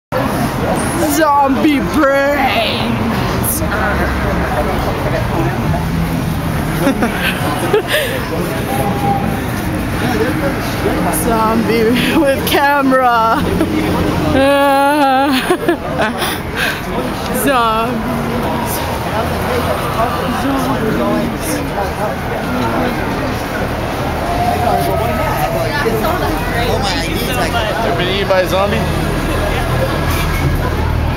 ZOMBIE WALK HOLLYWOOD 2009
heather oakes angel oakes and david gomez at the zombie walk lookin for BRAINS!